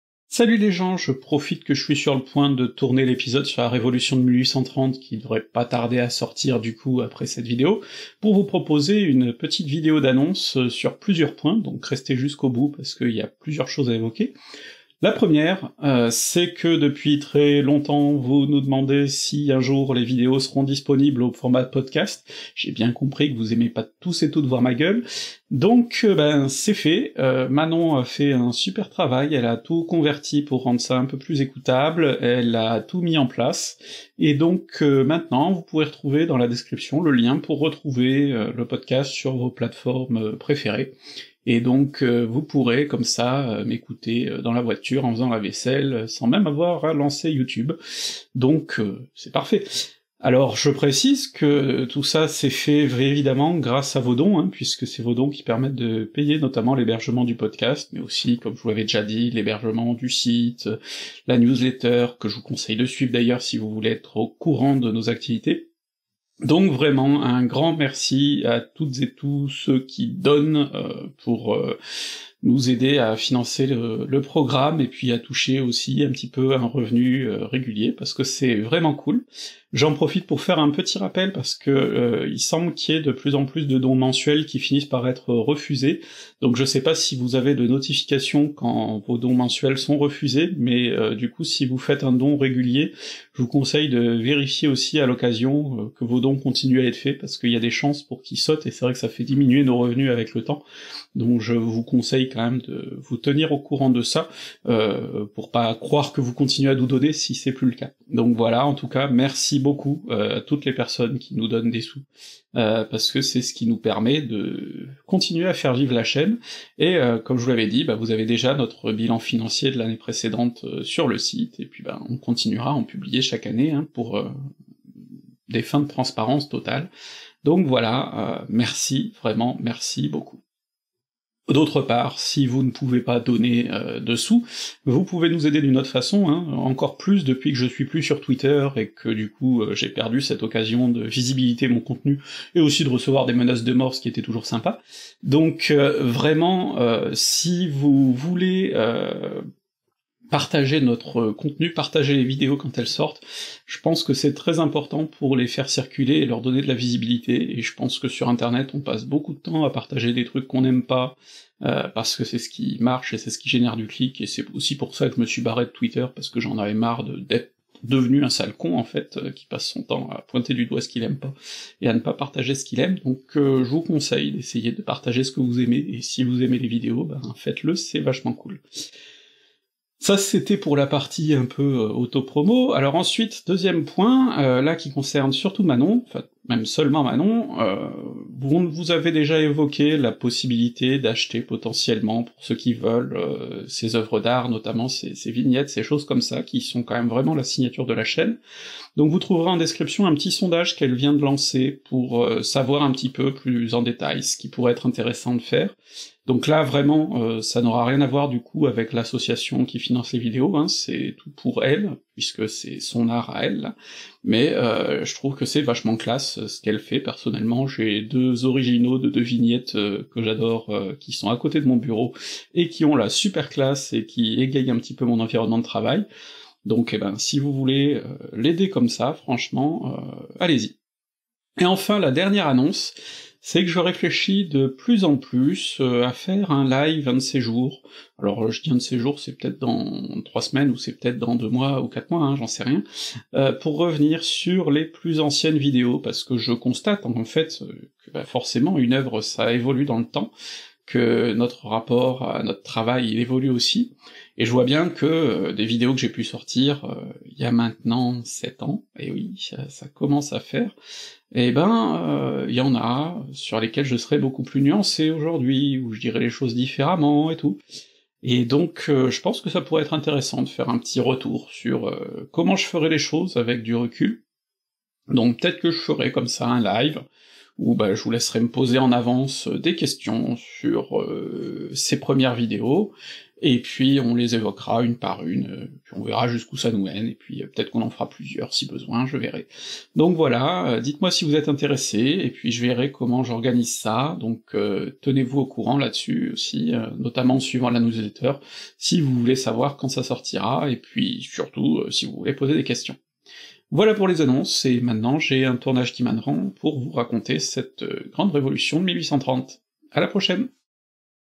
Les podcasts d'Histony sont disponibles !
Les vidéos de la chaîne sont enfin disponibles en podcast pour les gens qui préfèrent n'avoir que l'audio ! On parle aussi de quelques autres projets futurs.

Une vidéo écrite et présentée par Histony ; montée, réalisée et illustrée par Loma, le tout sous licence CC-BY-SA.